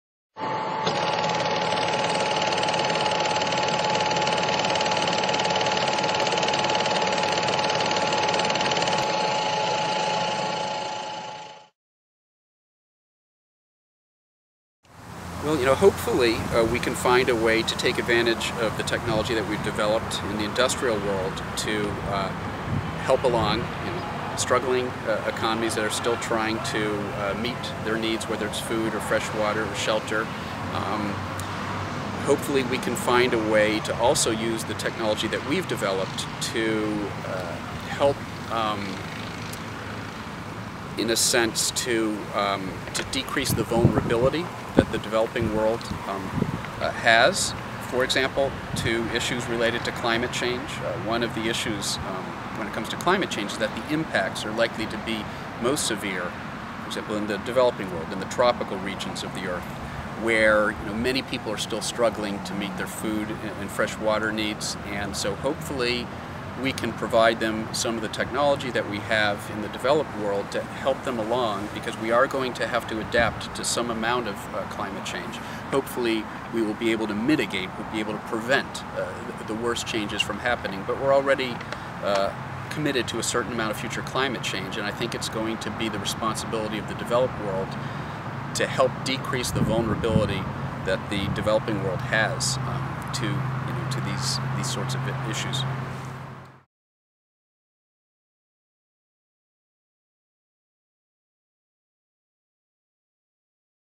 Michael Mann on technology and developing countries
Climatologist and geophysicist Michael Mann spoke with the Weave in Canton, NY in September 2012.